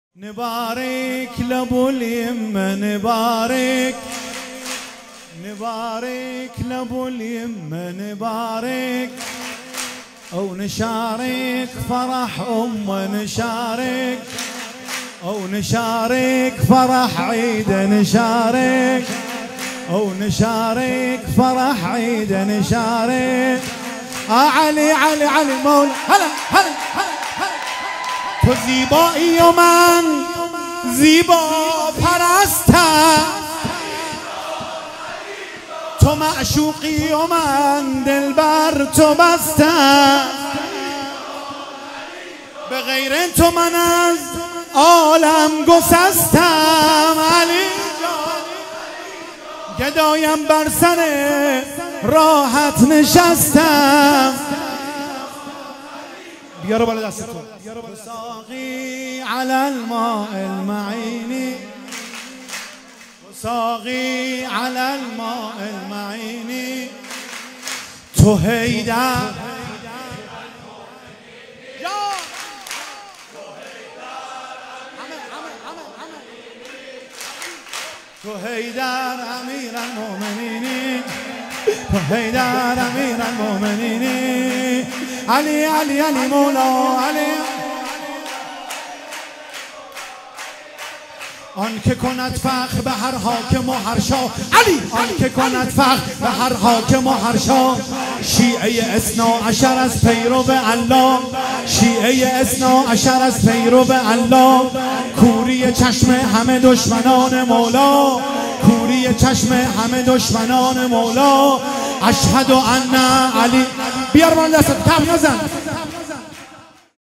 مداحی ملا محمد فصولی عید سعید غدیر خم هیئت جوانان کربلایی تهران
مداحی ملا محمد فصولی عید سعید غدیر خم هیئت جوانان کربلایی تهران تهیه شده توسط خانه هنر پلان3

پیام رسان

ادمین تلگرام

شماره تماس

Messenger

@ PELAN3
Telegram admin

Phone number